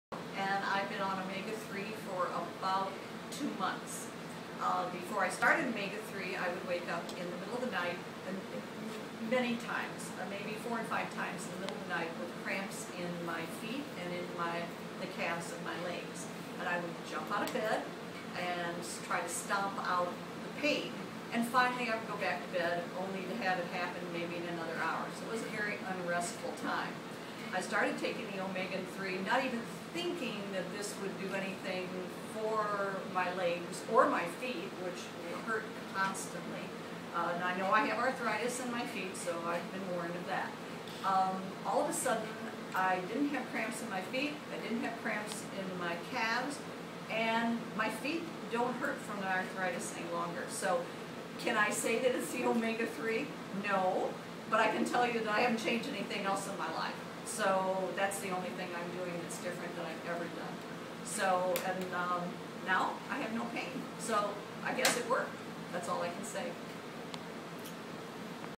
Judy's testimony about her experience with the Marvalous Omega 3 - NO MORE CRUMPS!!!
Artritis, crumps are gone from the Marvalous Omega 3!!!